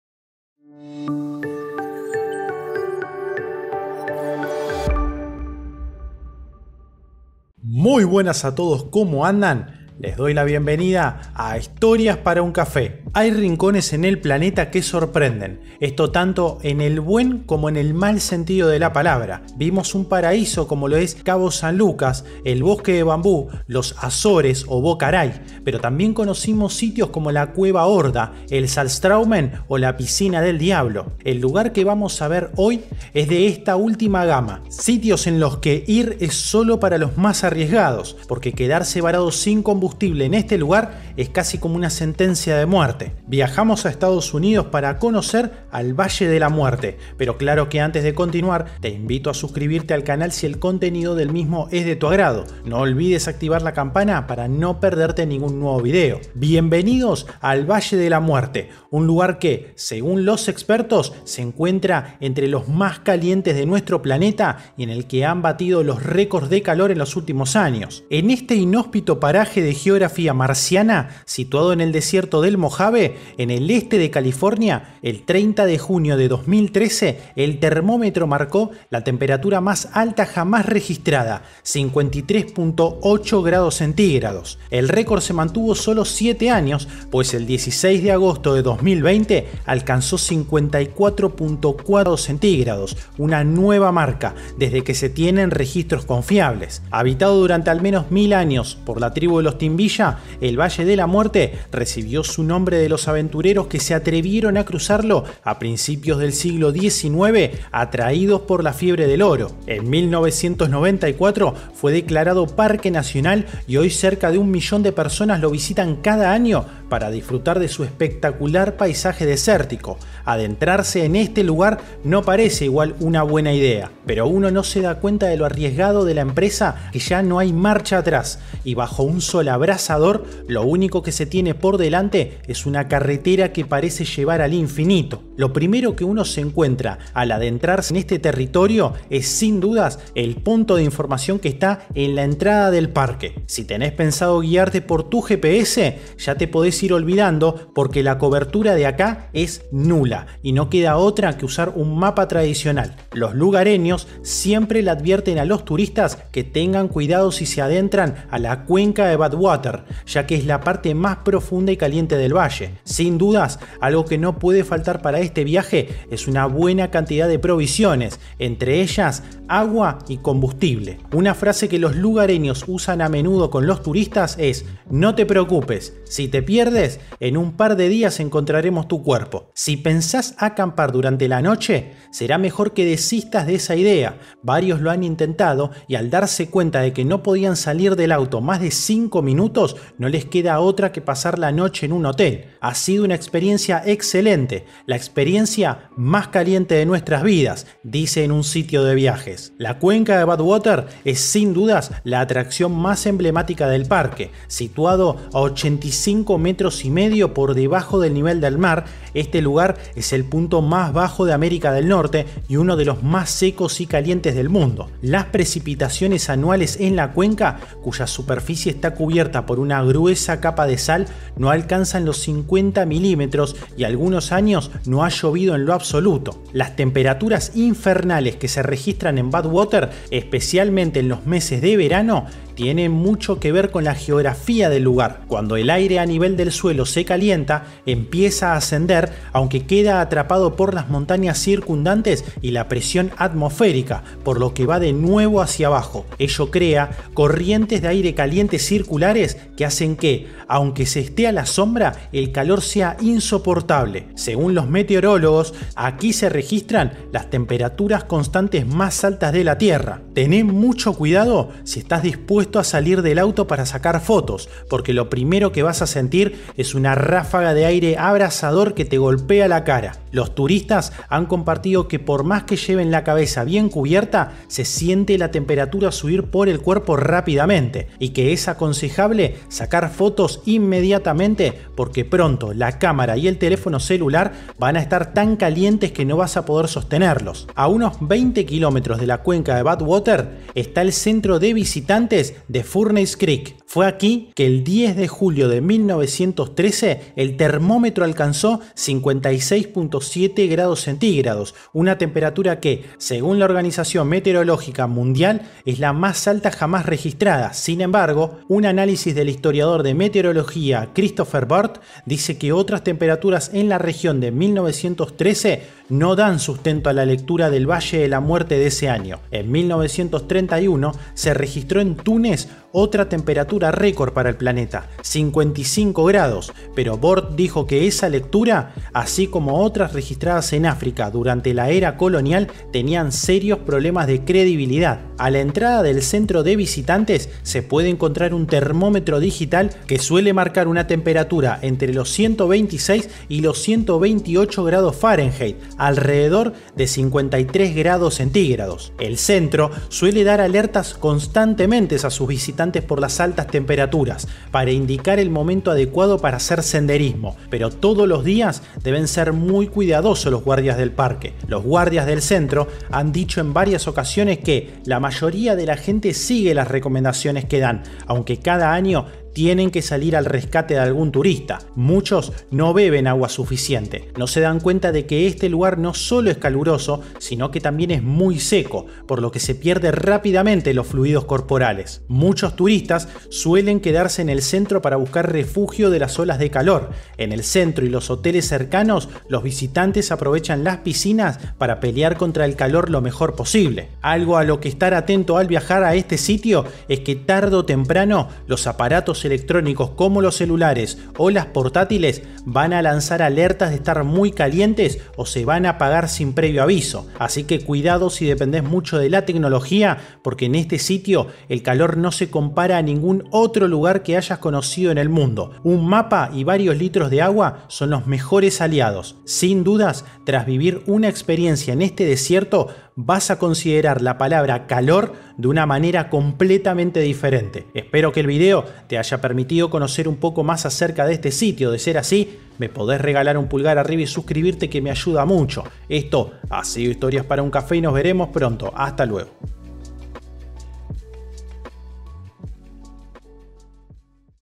El Valle de la Muerte California - El Lugar mas Caliente del Planeta
Cuando uno busca records mundiales, uno de ellos es el punto de la tierra con la temperatura más alta de todos y eso nos lleva al Valle de la Muerte en California, nombre bien puesto por cierto, porque describe perfectamente este paraje inhabitable.